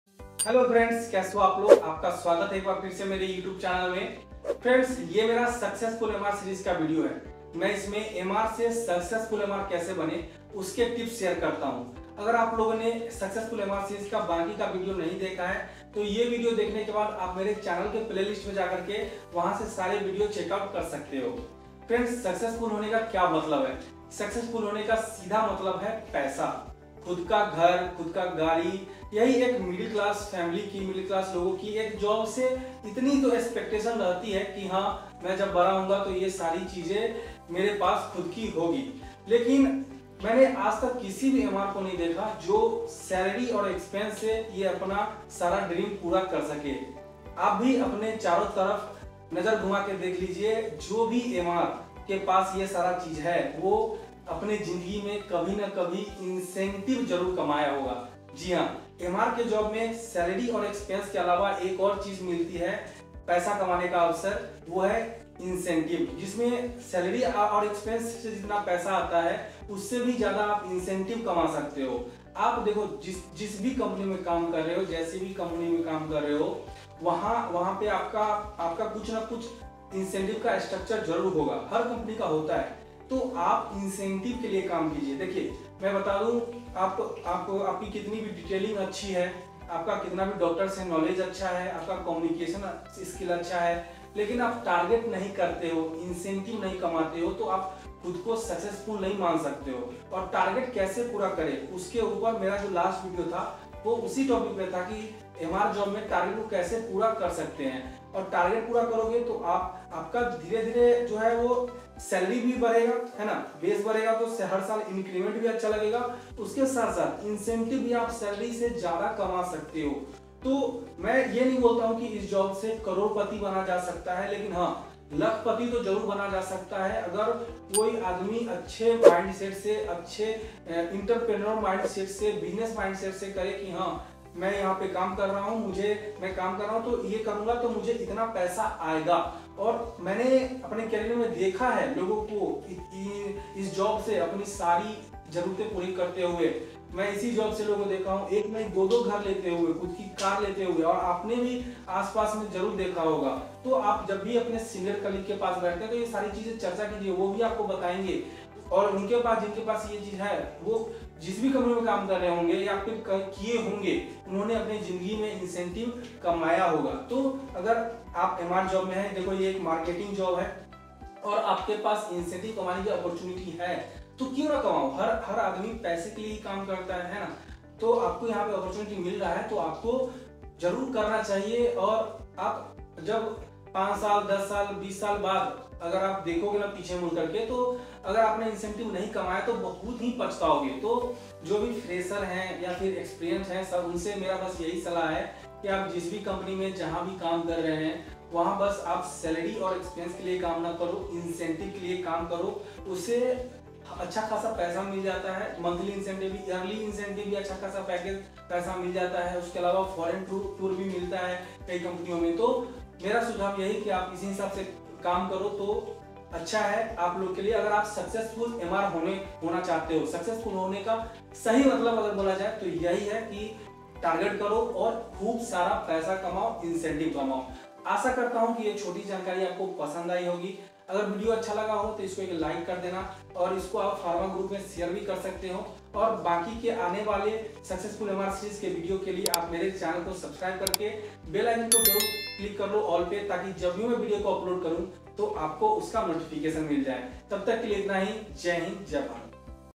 MR job से लखपति | Medical Representative Salary | MR Total income | SMR-9 | Anmol KT | LOMRSP-122
Medical representative salary
MR job से लखपति

Friends this is my successful MR series video in this video I have explained about how can you earn in Lakhs in medical representative job

For more MR job related videos check out my YouTube channel